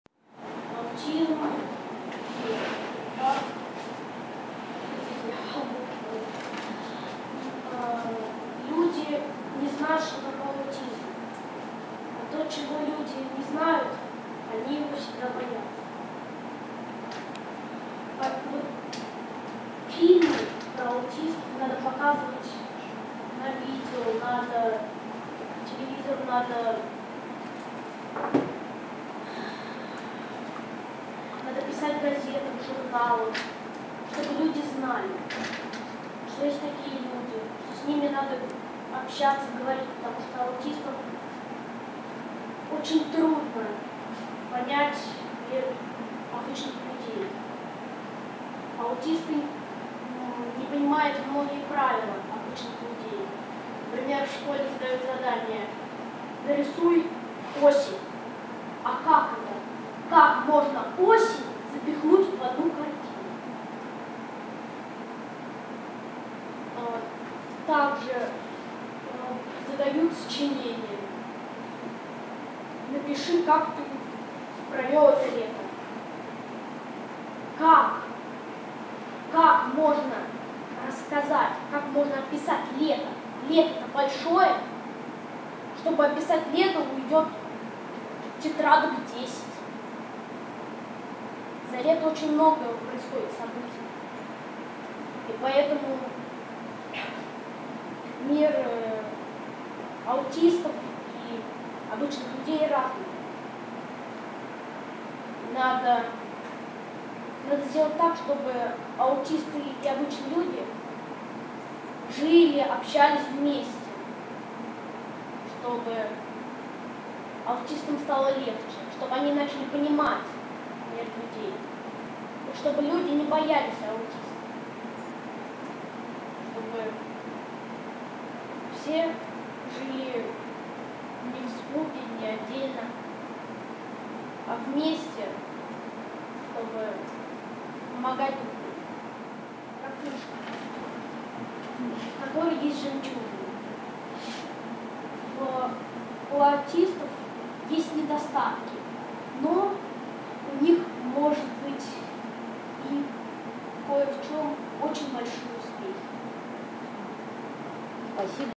Мальчик-аутист о том, почему не надо бояться таких, как он
Здоровые люди не должны бояться аутистов. Такое мнение сегодня на заседании круглого стола, посвященного проблемам адаптации в современном обществе аутичных людей, высказал ребенок, принадлежащий к этой категории, Герман Рудов.